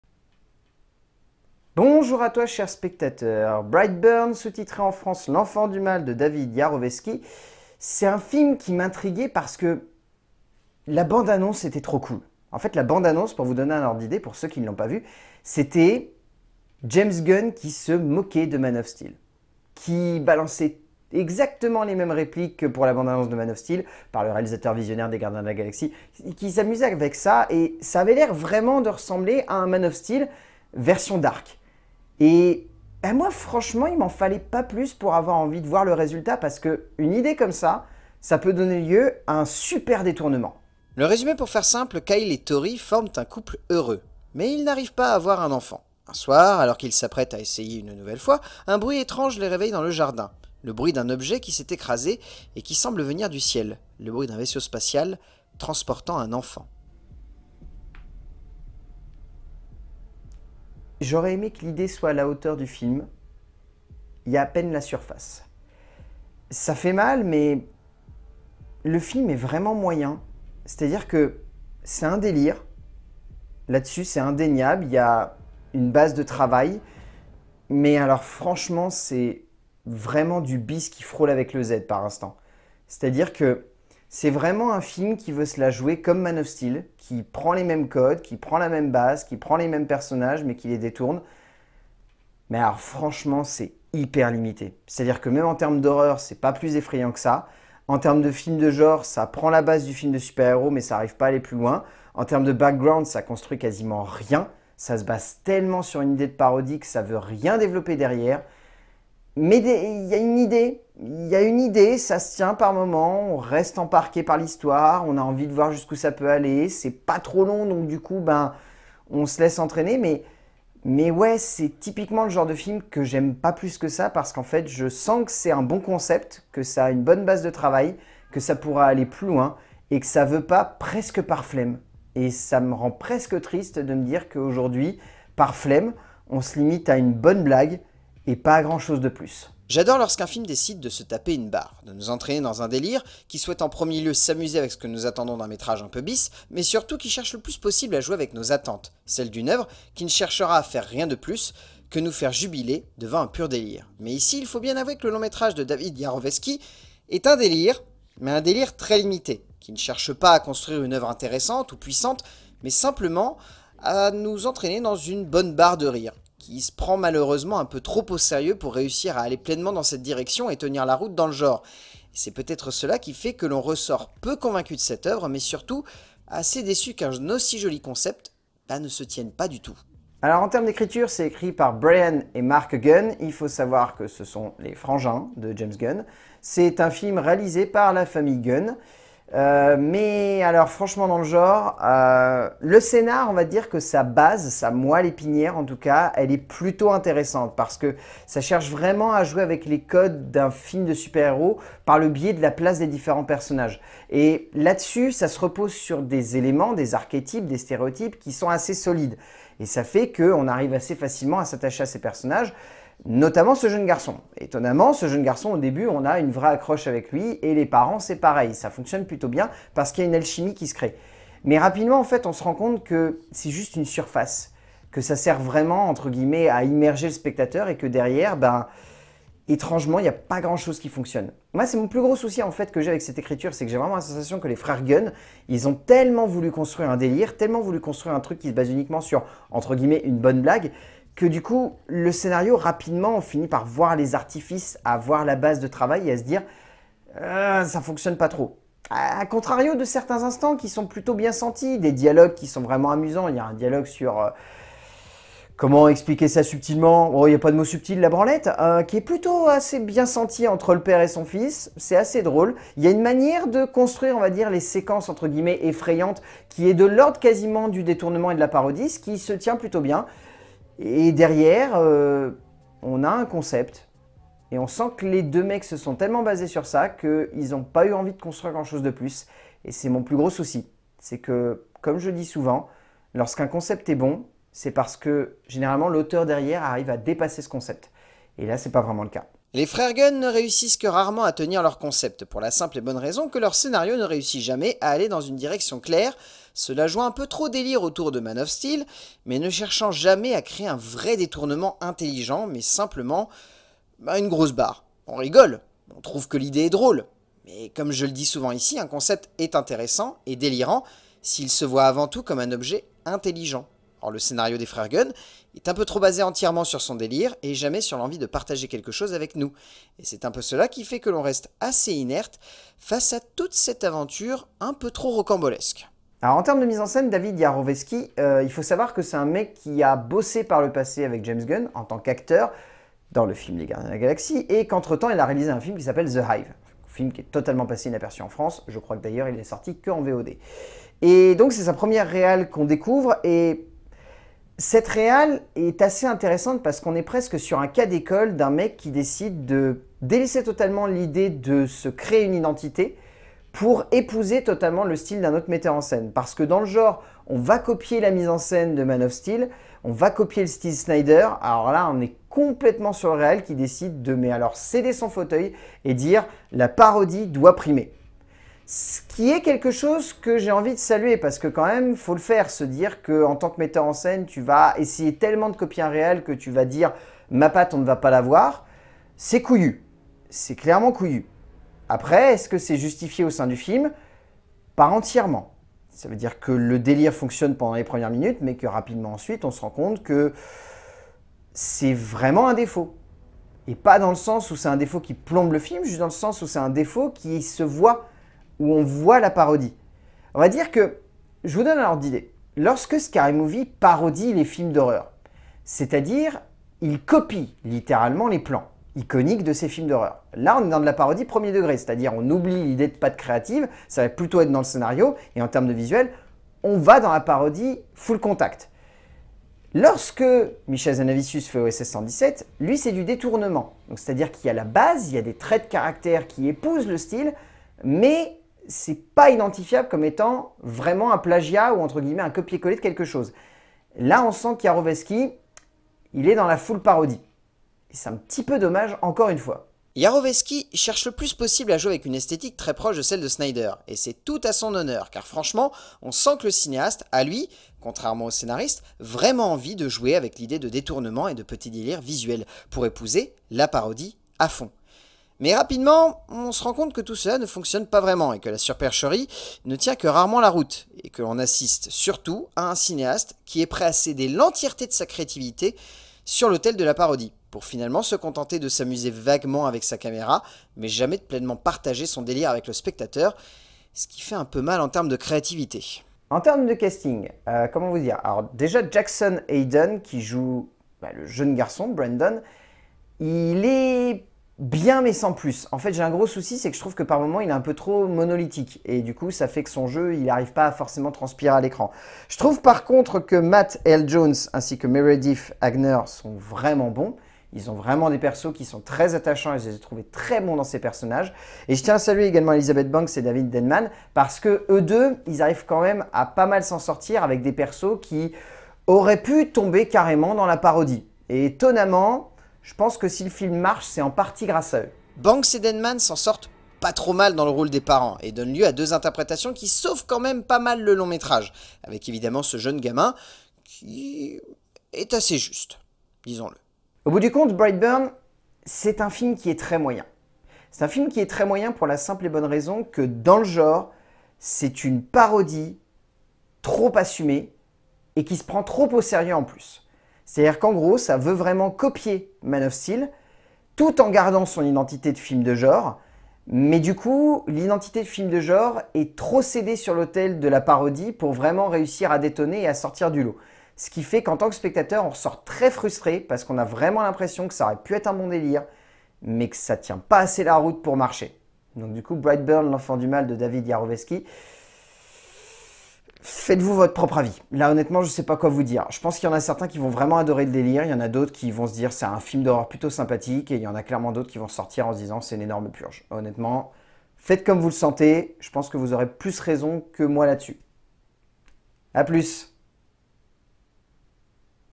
LCDC - Brightburn, l'enfant du mal
Chronique critique sur la sortie d'un film d'épouvante, plutôt à classé dans la série film de série B, autour d'un enfant, et d'une parodie peu subtile, "Brightburn, l'enfant du mal"